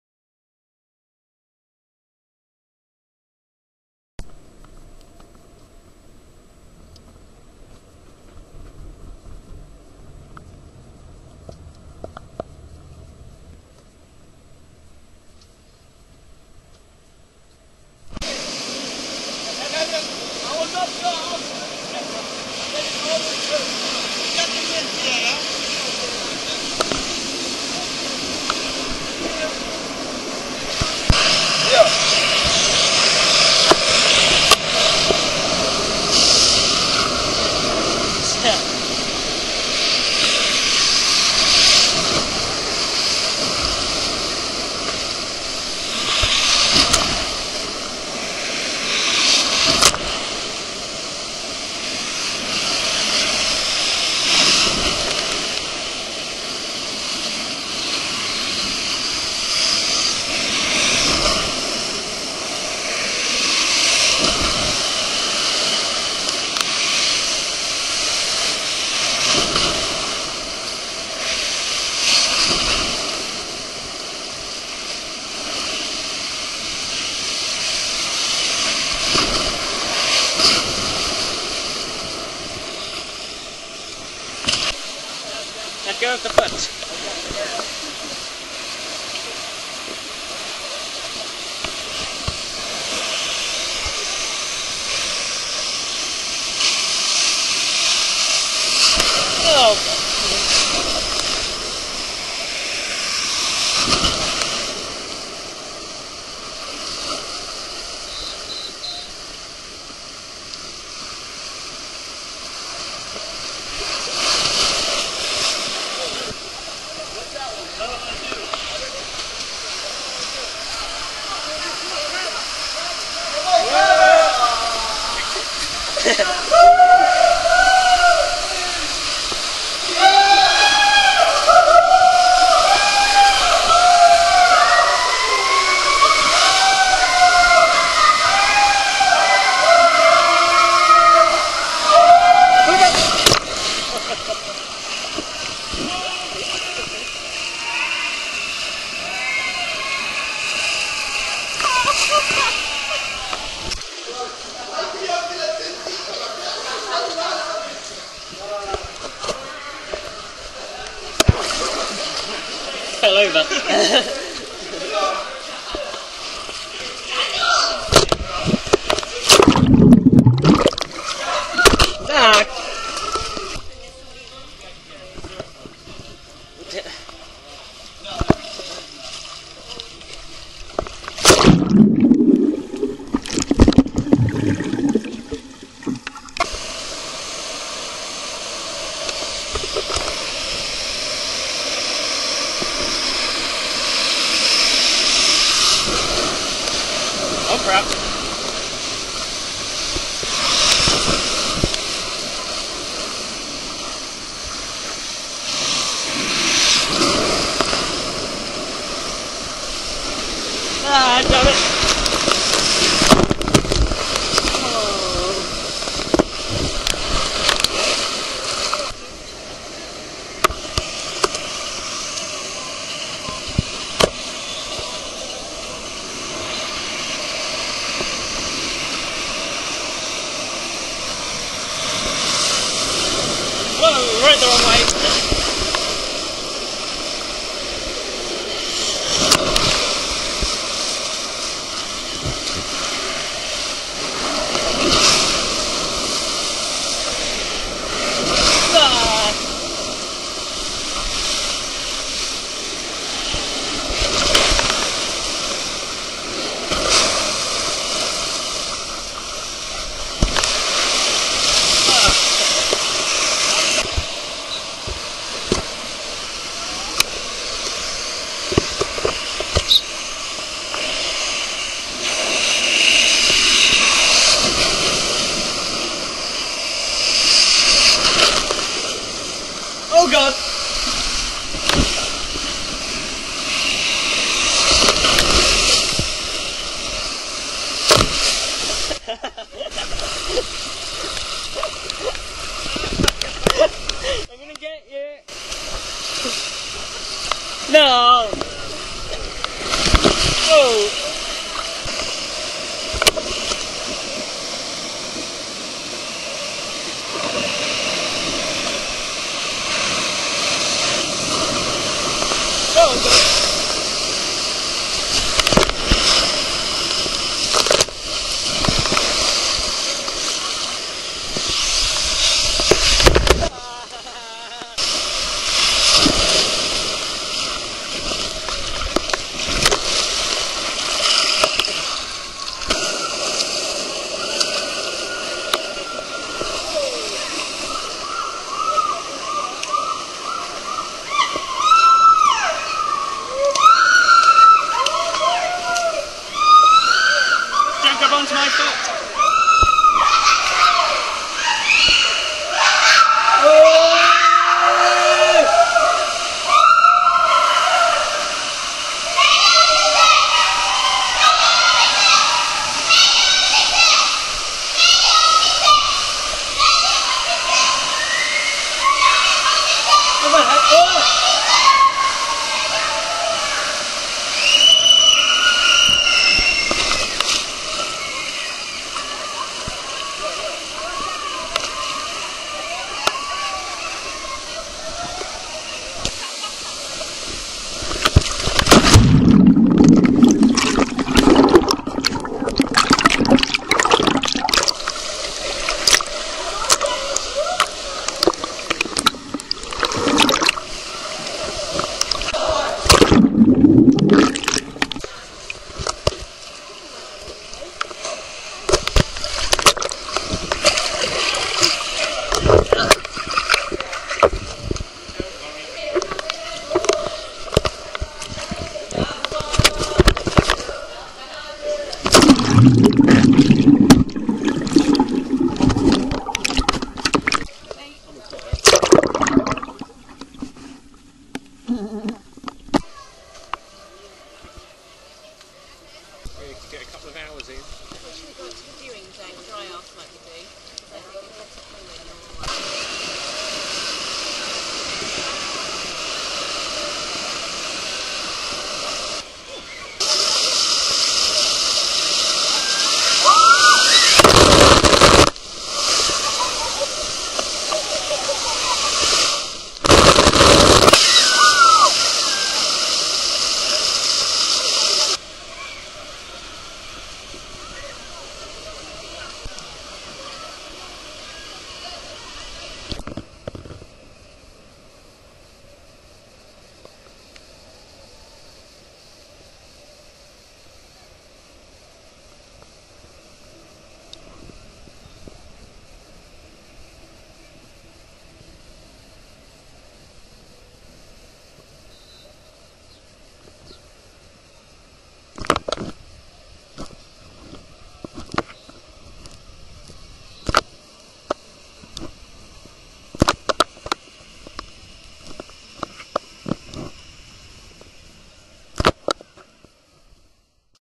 Wild Wadi Waterpark - Filming On Rides
Filming all of the rides in Wild Wadi Waterpark in Dubai.

my new videos come out.
Also like and favourite the video as it
Also like and favourite the video as it can help me out a lot!